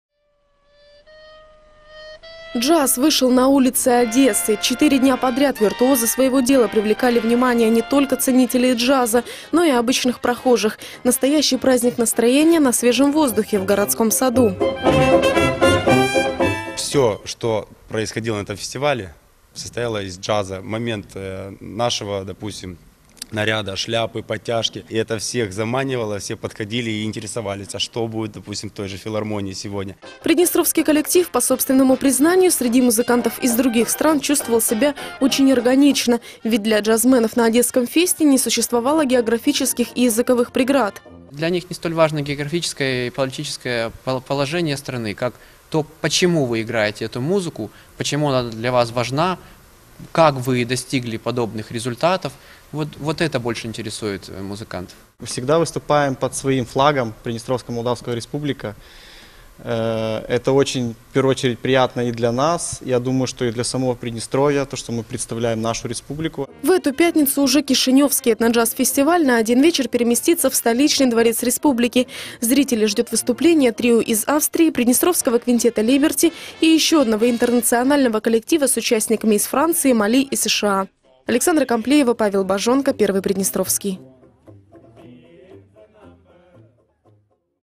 Коллективы из 15 стран мира собрались в Одессе на джазовом фестивале
Праздник джаза. Коллективы из 15 стран мира: от Кубы до Канады, от Индии до Великобритании собрались в Одессе на джазовом фестивале. Среди участников был и приднестровский квинтет «Либерти».